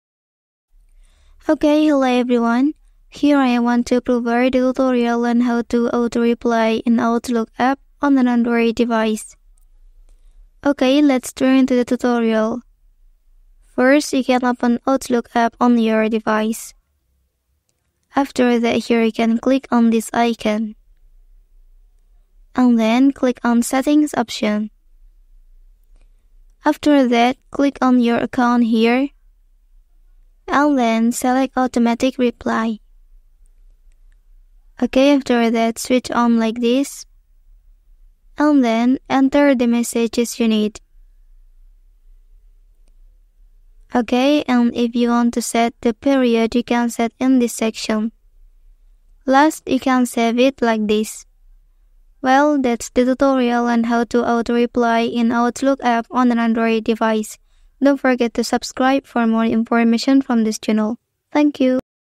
How To Auto Reply On Outlook App Android
How to auto reply on outlook app android - Like other email apps, Outlook has an automatic message reply feature with a time that you can determine. You can schedule when messages will be answered automatically, for example when you are traveling or on holiday.

how to create auto reply on outlook.